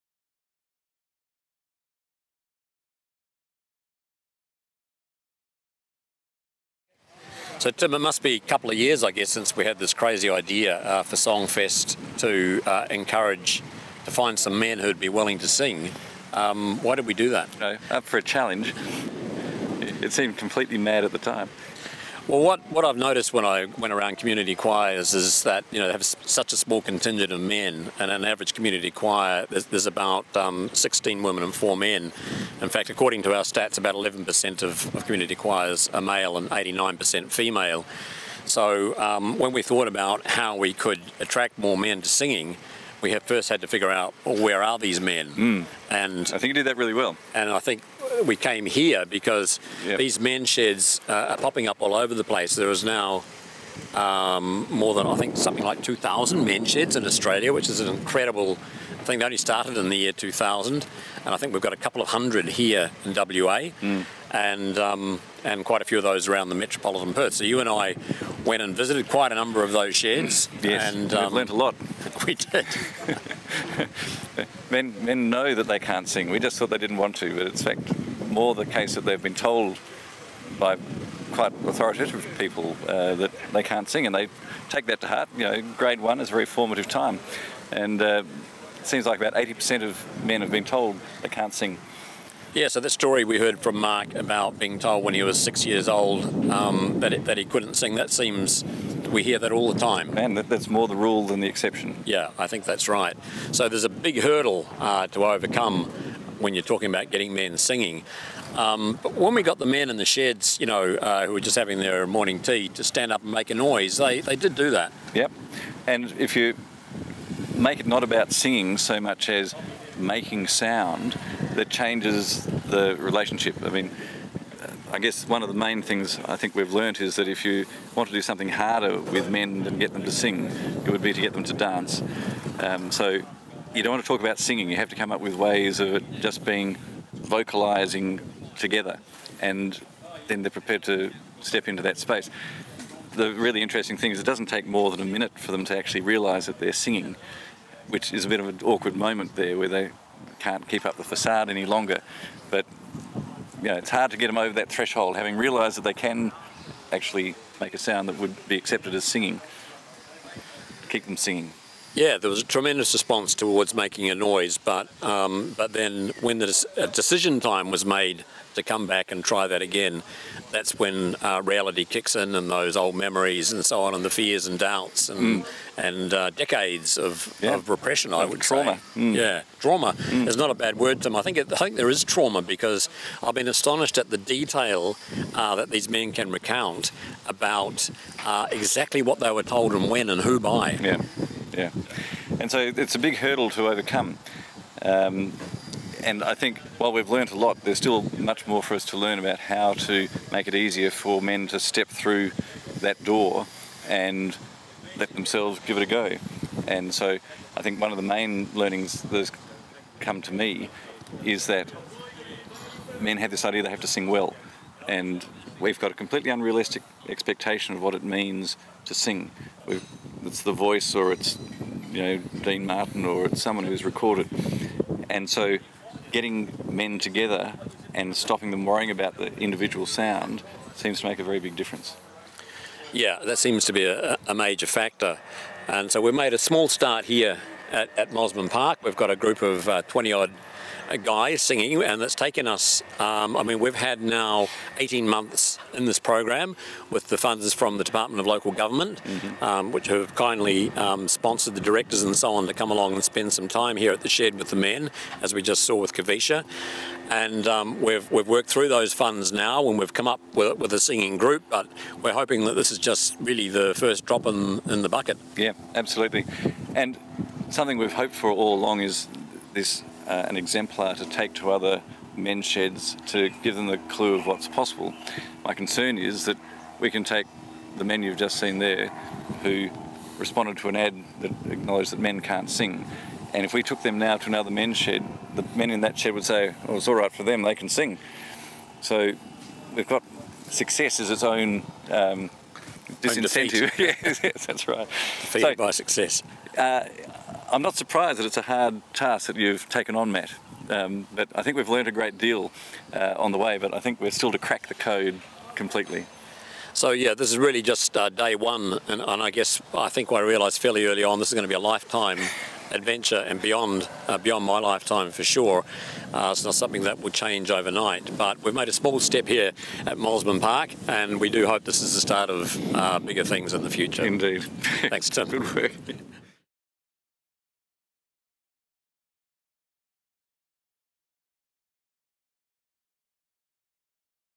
ShedSong Interview #5
SHEDSong MEN'S SHED Mosman Park Interview with Tim Fisher & Matthew Clements

Credits

SongFest Inc. | ShedSong Choir Director - Tim Fisher
SongFest Inc. | ShedSong Program Director - Matthew Clements
Musician | Guest Choir Director - Kavisha Mazzella
Videographer - Maree Laffan Photography